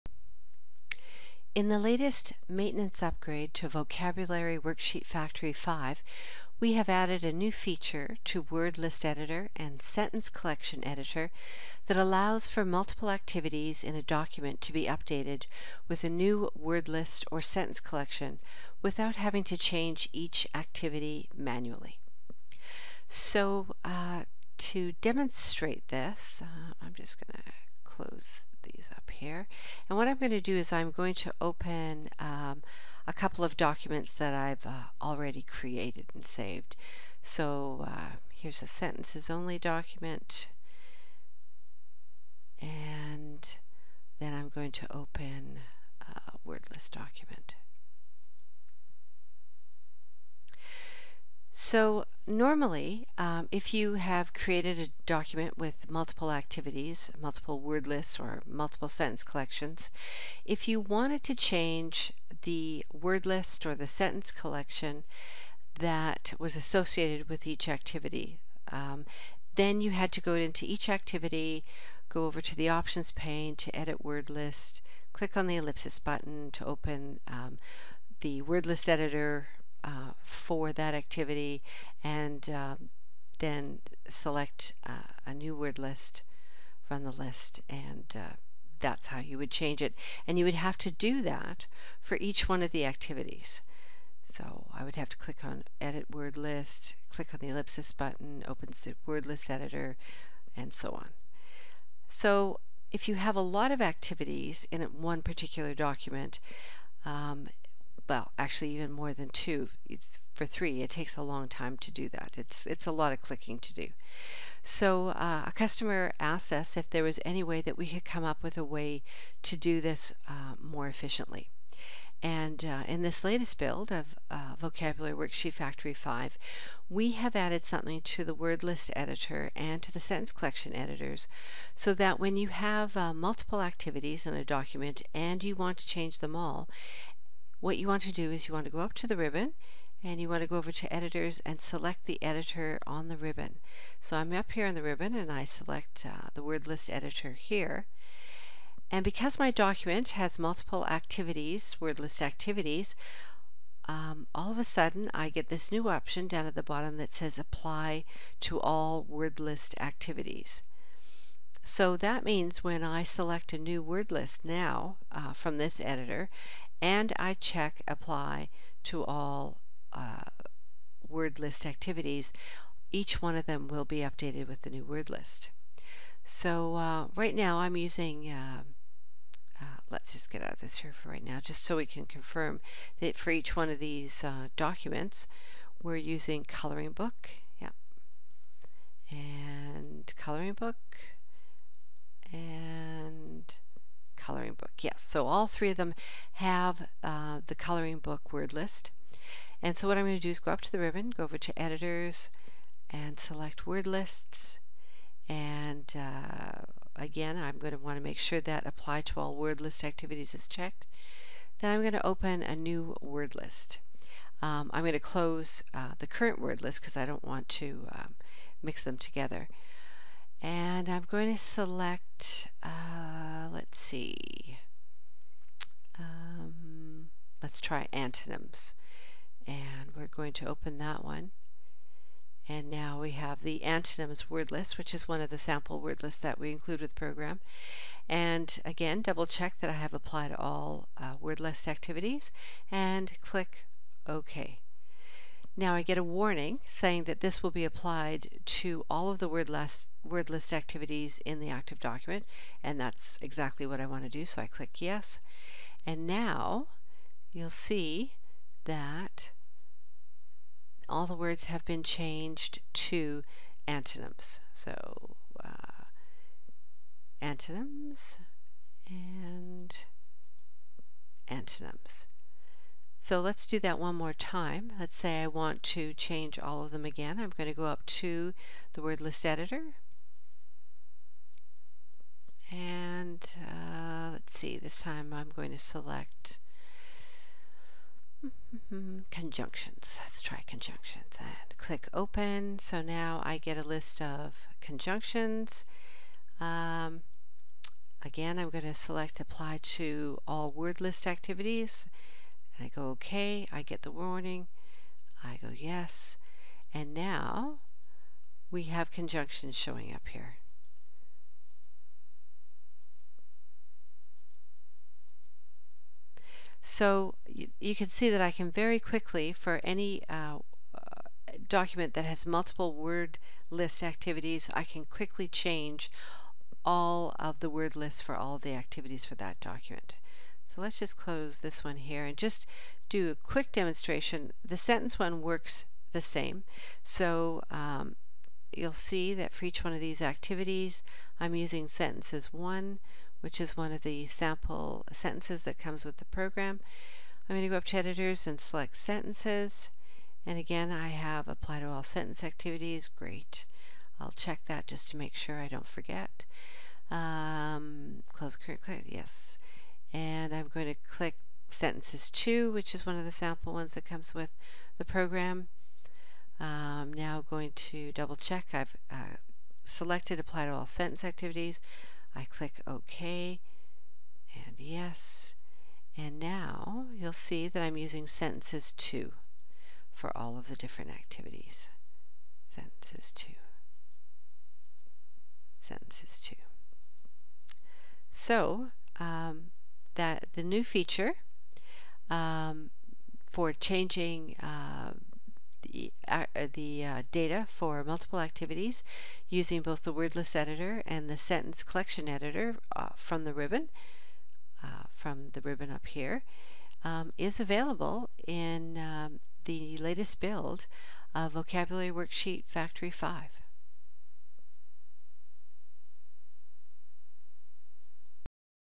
Vocabulary Worksheet Factory 5: Change Data for Multiple Activities
Apply a new word list or sentence collection to all activities in a document.